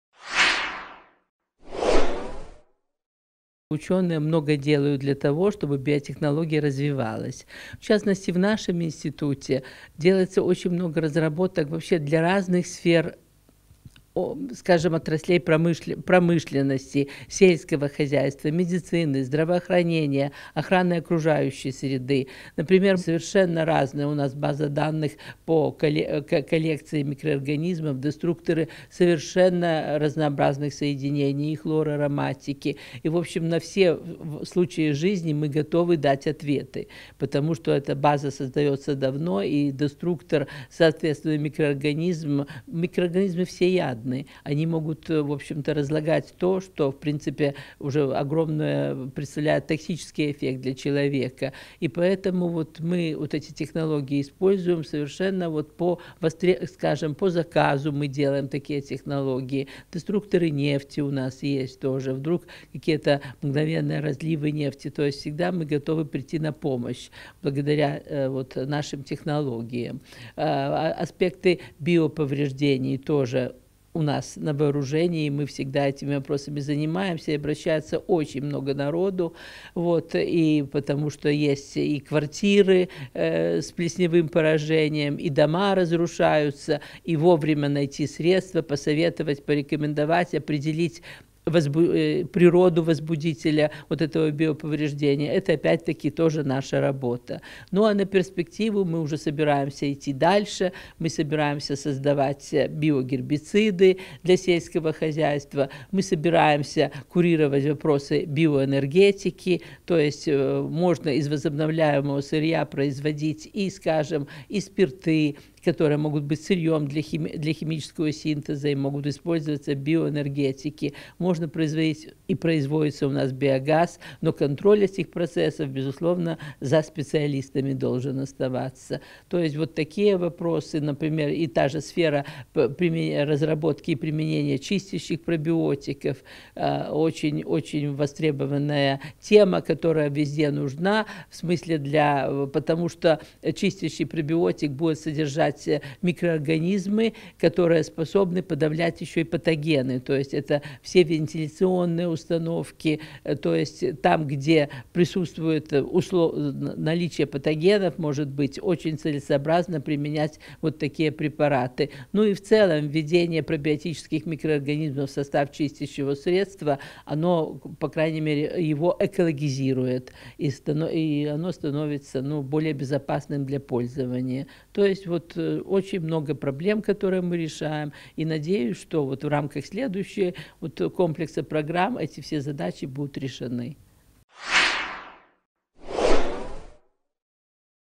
Достижения белорусских ученых в области биотехнологий
Директор ГНУ "Институт микробиологии" НАН Беларуси Эмилия Коломиец  в пресс-центре БЕЛТА рассказала о достижениях белорусских ученых в области биотехнологий и сферах их применения.